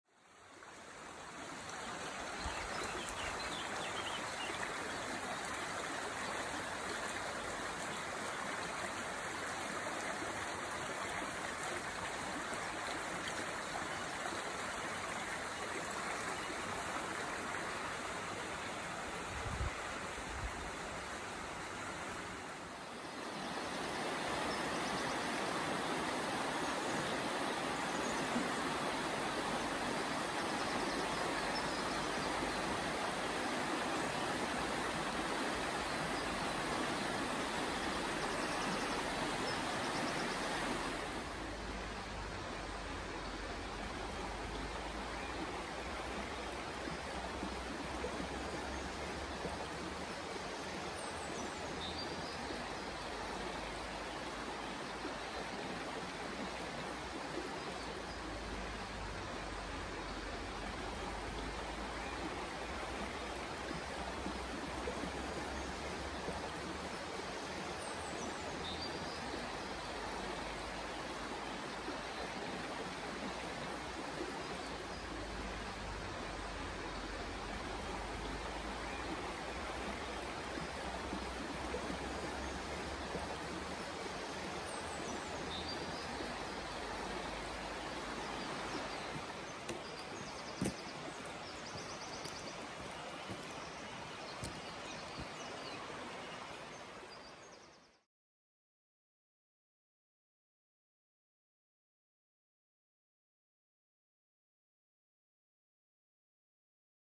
The River Camel At Gam Bridge : Tuckingmill, St Breward
The River Camel at Gam Bridge, Tuckingmill near St Breward, earlier today.  Relaxing sounds of the flowing river . . . and you might just hear the chorus of birds busy chirping away in the background!